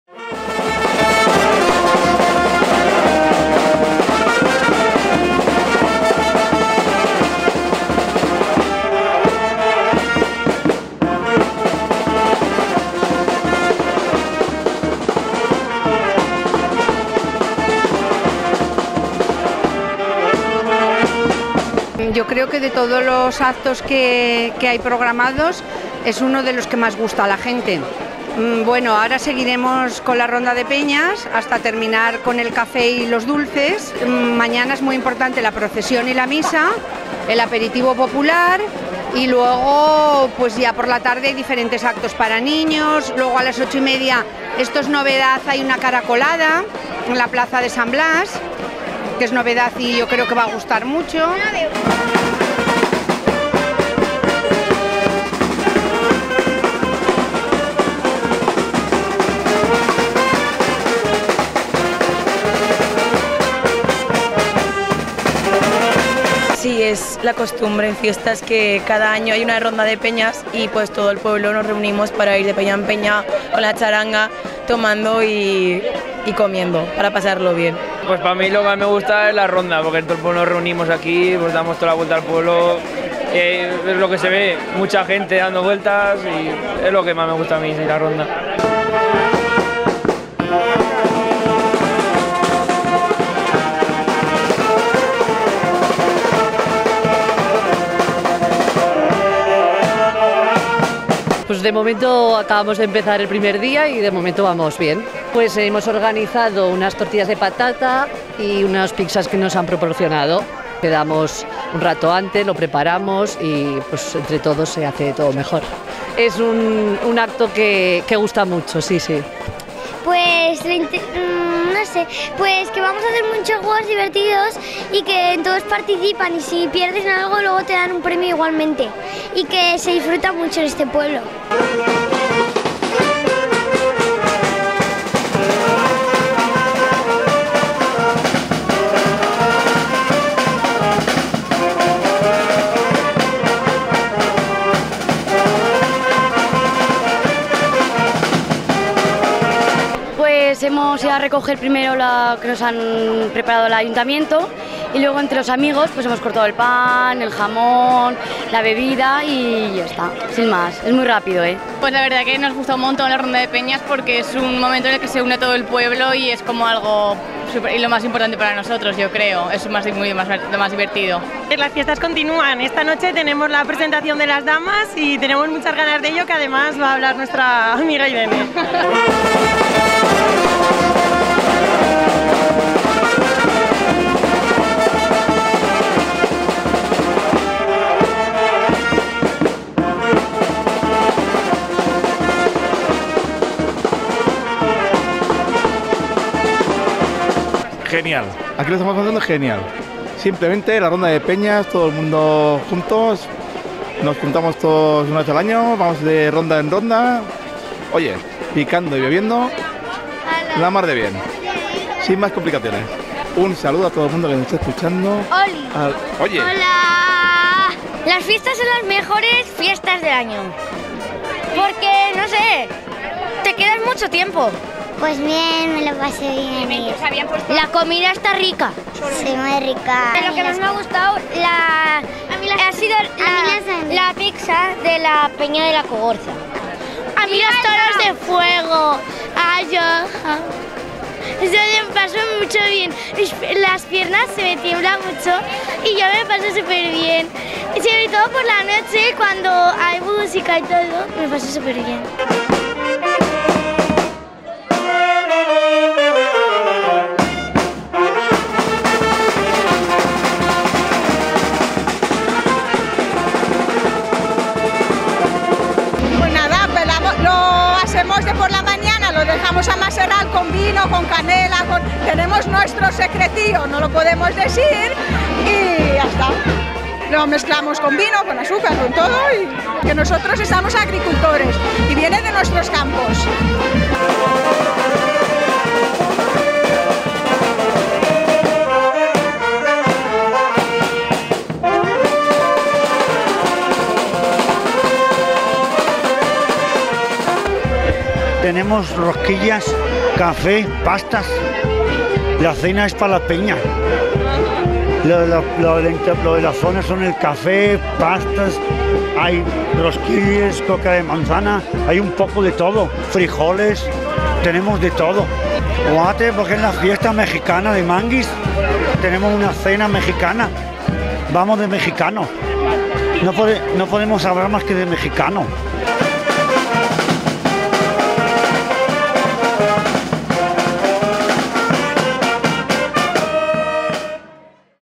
Ronda Peñas Chiprana 2018
El acto más multitudinario, el acto más esperado y el más sabroso de las fiestas de Chiprana. La ronda de peñas recorrió a ritmo de charanga las calles del pueblo y las peñas prepararon merienda para todos. Jamón, pizza, anchoas, embutidos, melocotón con vino, pastas, café. El acto preferido de todos los chipranescos.